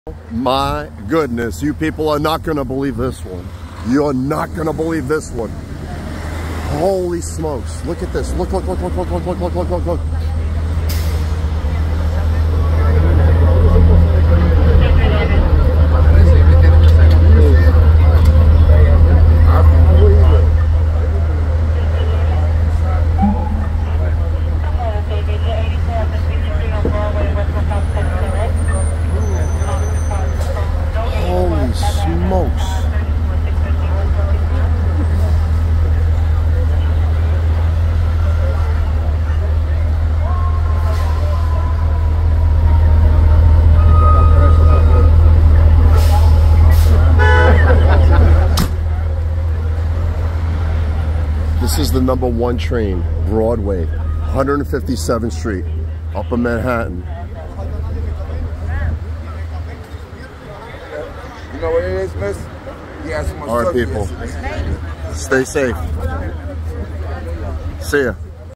Driver Crashes Into Manhattan Subway Stairwell
A 25-year-old man was arrested after crashing a vehicle into the stairwell entrance to a subway station in Upper Manhattan on Friday, November 24, local media reported.